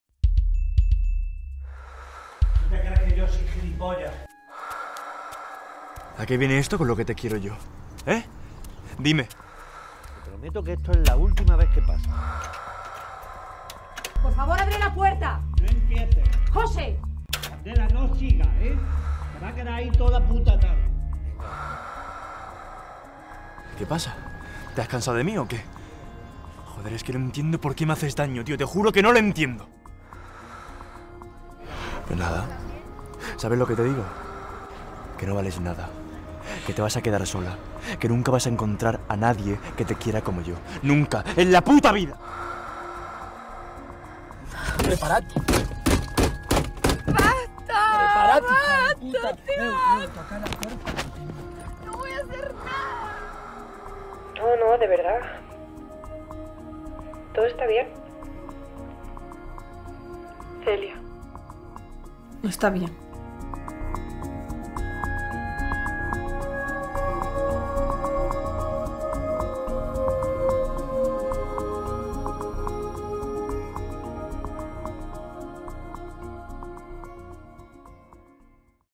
25N \ Día Internacional contra la Violencia de Género
💜 Si escuchas esto, implícate y ACTÚA. No mires hacia otro lado.

Para acabar con la ViolenciaDeGénero tenemos que actuar, pero ante todo debemos educar en la igualdad y el respeto.

♀️ Por un presente y un futuro sin esta lacra, porque